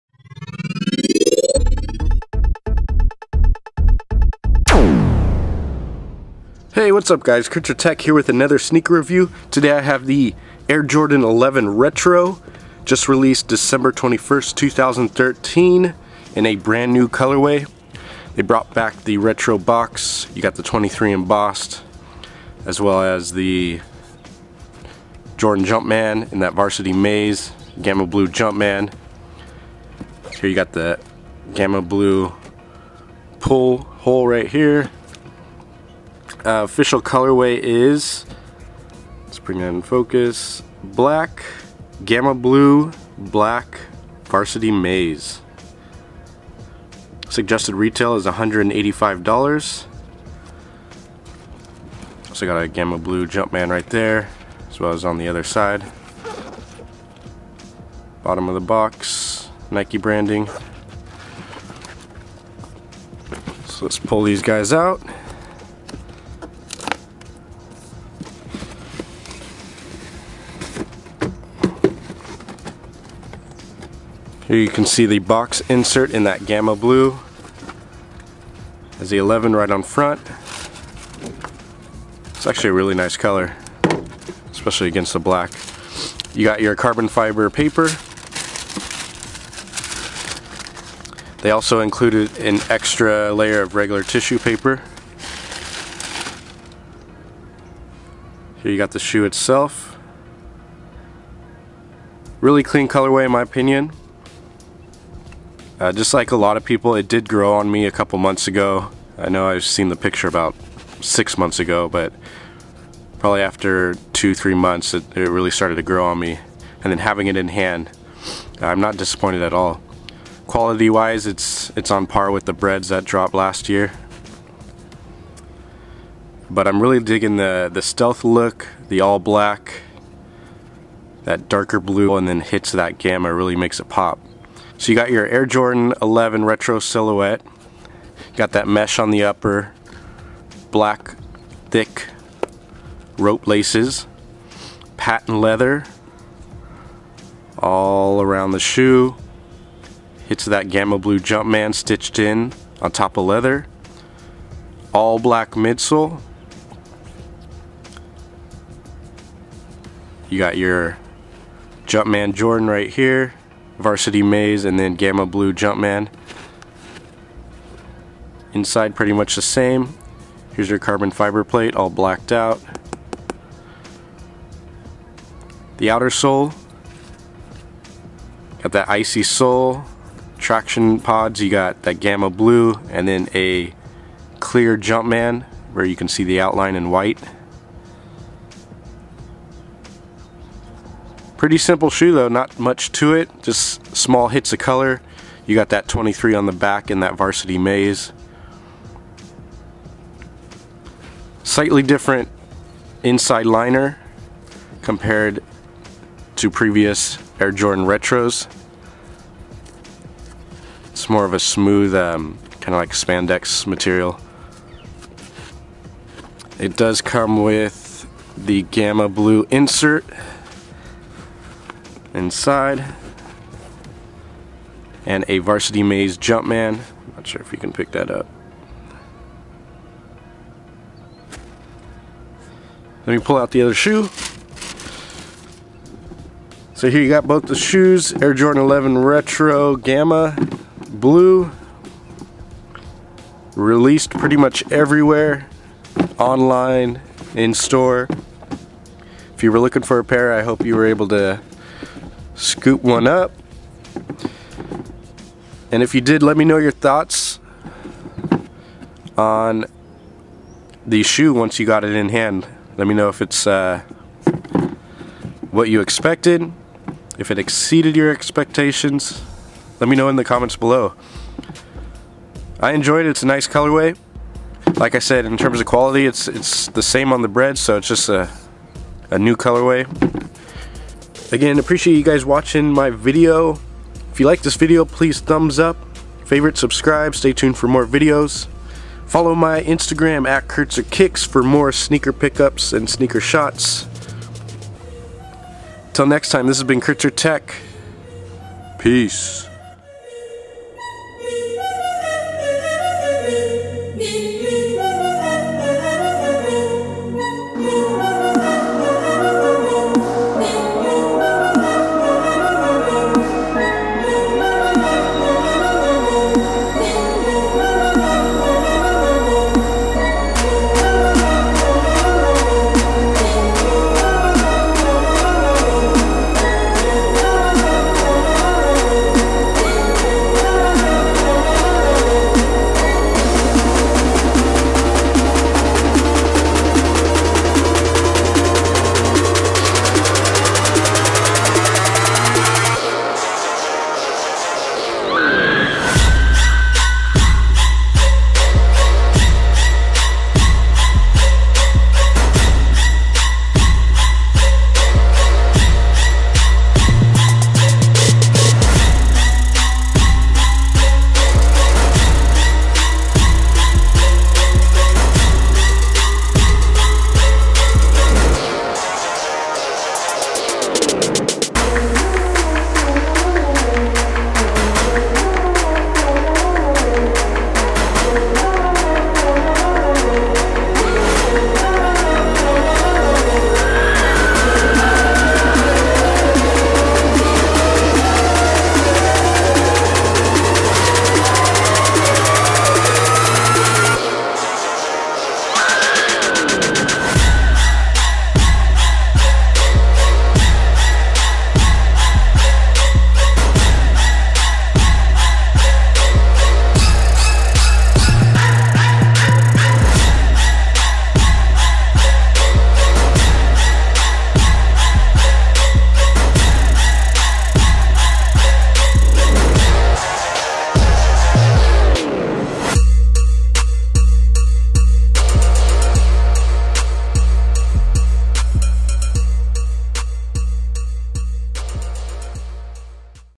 Air Jordan XI 11 Retro "Gamma" (on feet)
A KurtzerTech review of the Air Jordan XI 11 Retro "Gamma".